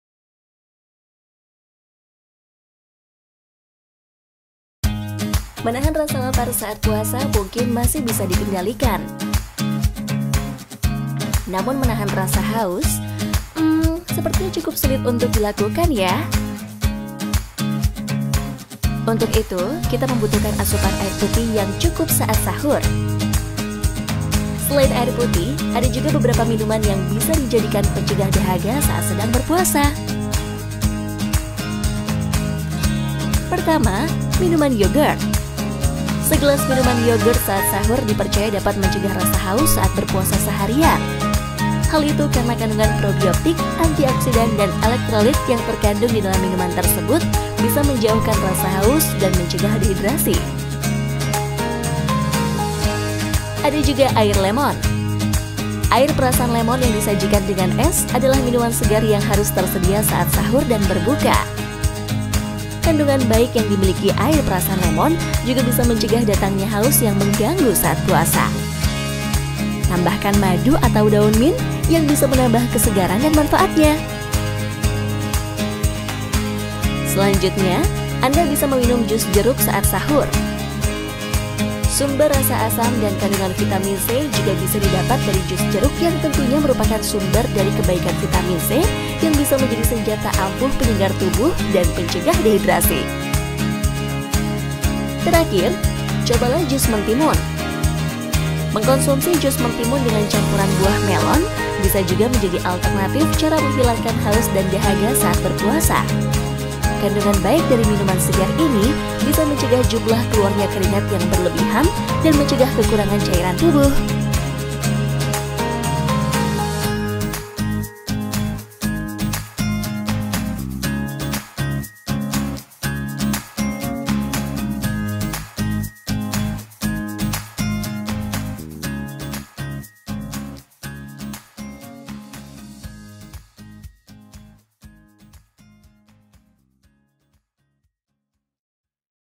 5 Minuman sahur yang ampuh cegah dahaga saat berpuasa
Menahan rasa lapar saat puasa mungkin masih bisa dikendalikan. Namun, menahan rasa haus sepertinya cukup sulit untuk dilakukan ya?

Untuk itu, kita membutuhkan asupan air putih yang cukup saat sahur. Selain air putih, ada juga beberapa minuman yang bisa dijadikan pencegah dahaga saat sedang berpuasa.

Berikut merdeka.com merangkum 5 minuman yang bisa mencegah dahaga saat berpuasa.

Simak videonya.